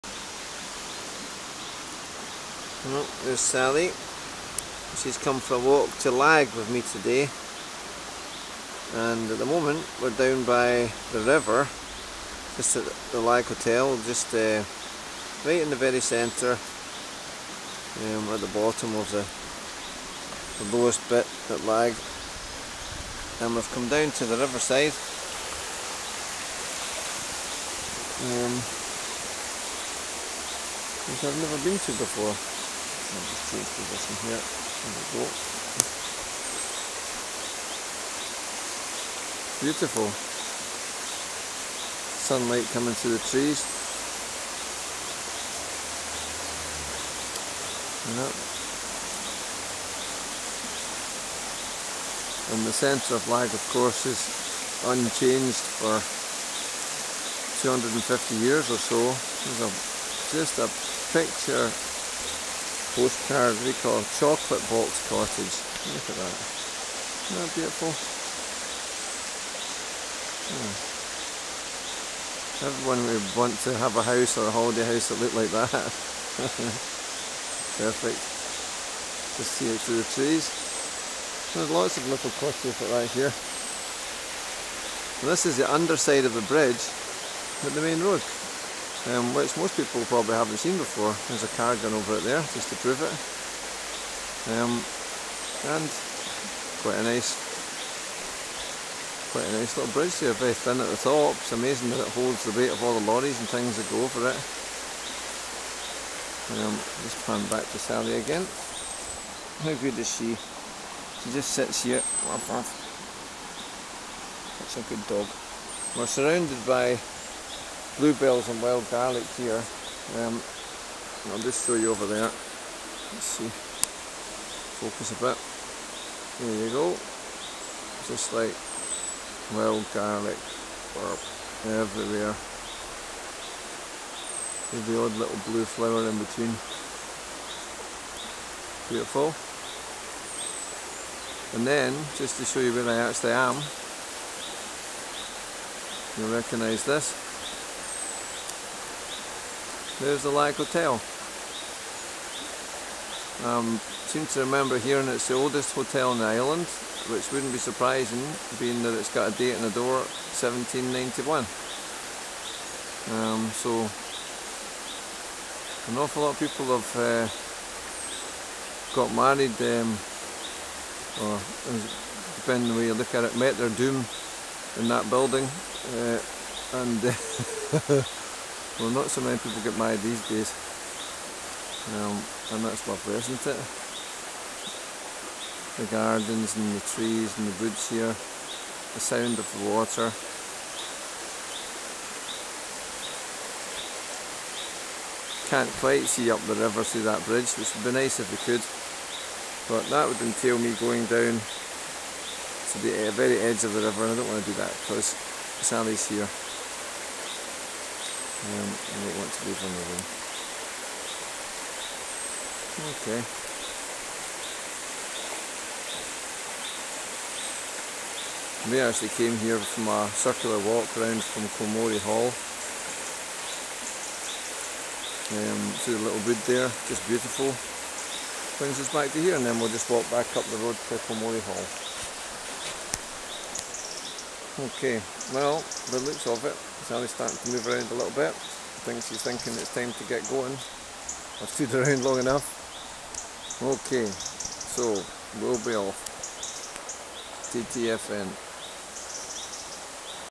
Lagg Isle of Arran@davetheraverovingtherock
Our wee ramble around Lagg this morning.
Sally The Teenage Westie & I started our walk at the Kilmory Hall and there's a lovely path there through the woods that comes to a junction, taking us either to the beach or else down to Lagg, which is where we went.
Last year I ran right round Arran unsupported by anyone else, and it was from the Lagg Hotel that I started and finished 9-and-a-bit hours later. 56@56 56 miles @ 56 years old :-)
This place therefore has bothvery  happy and very painful memories for me lol@davetheraverovingtherock